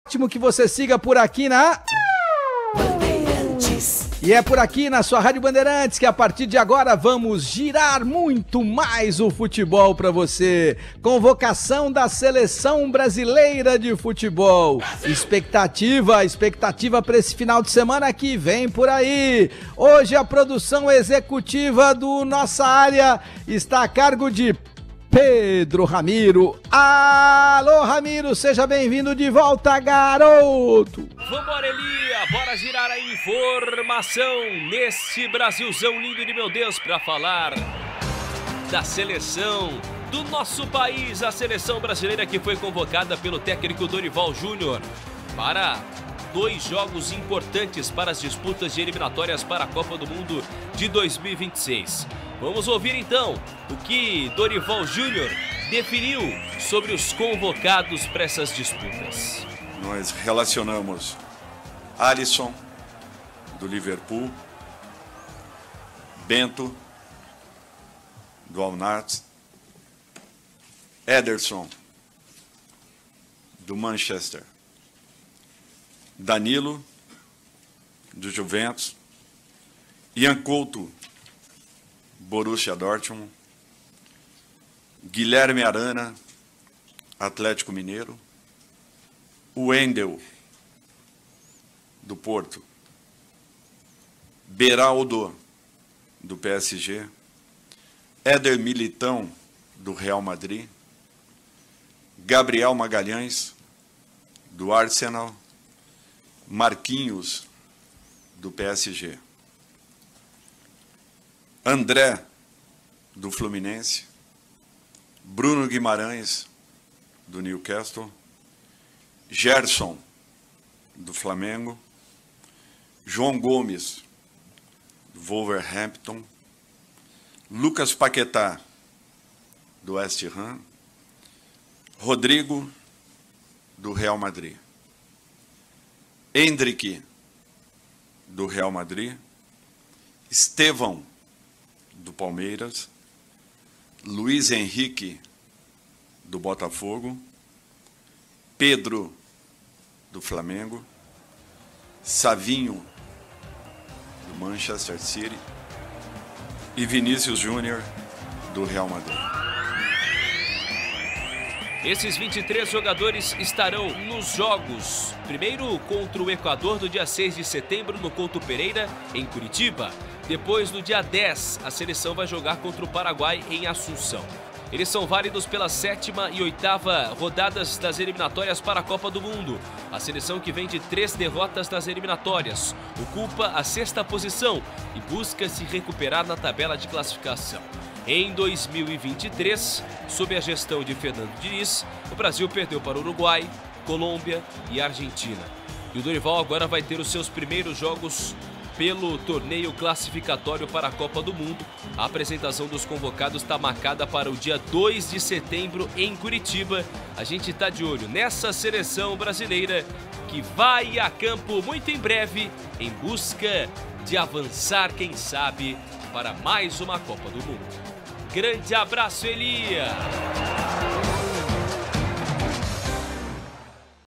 Dorival anuncia os convocados da Seleção Brasileira
Dorival anuncia os convocados da Seleção Brasileira. O técnico da seleção brasileira, Dorival Júnior, acaba de anunciar a lista dos jogadores convocados para os compromissos das Datas Fifa de setembro, contra Equador e Paraguai.